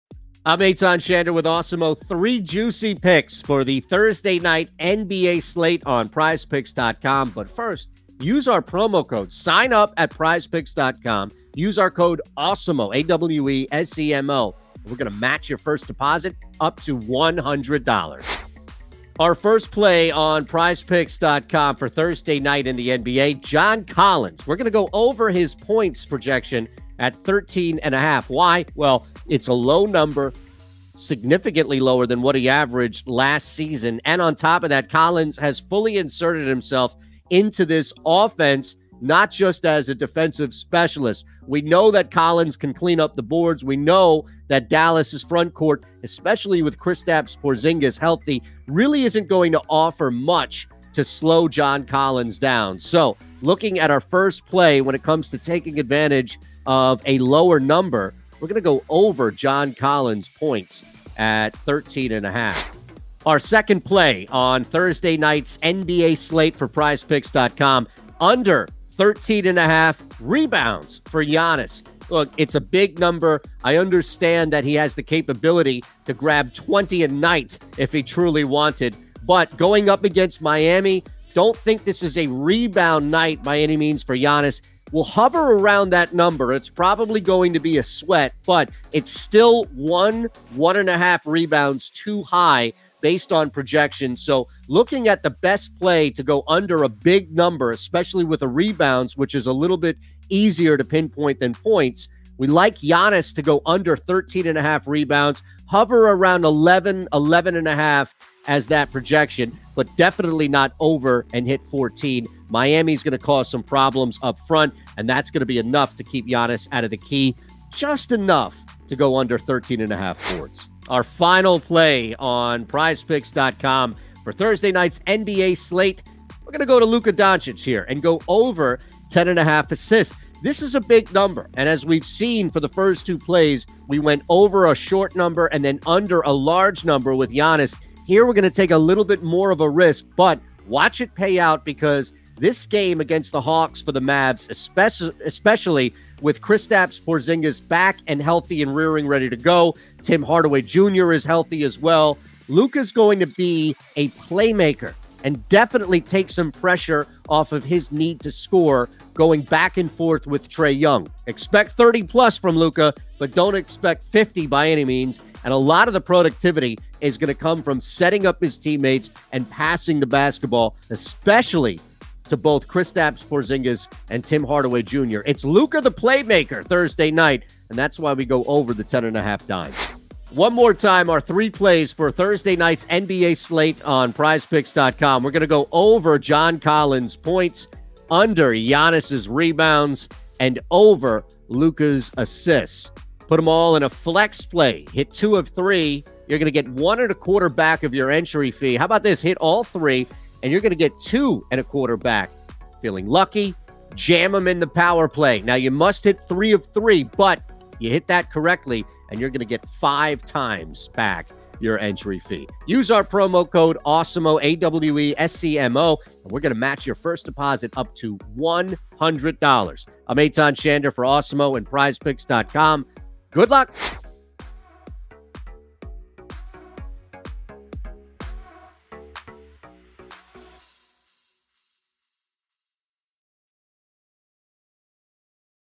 PrizePicks NBA Strategy & Lineups Tonight 10/21/21
Eytan Shander breaks down the NBA DFS slate and gives his favorite PrizePicks for daily fantasy basketball lineups tonight, Thursday, October 21.

SHOW INDEX
0:00 NBA PrizePicks Preview
0:19 John Collins OVER 15.5 Points
1:03 Giannis Antetkounmpo UNDER 13.5 Rebounds
2:01 Luka Doncic OVER 10.5 Assists
3:01 PrizePicks Recap Tonight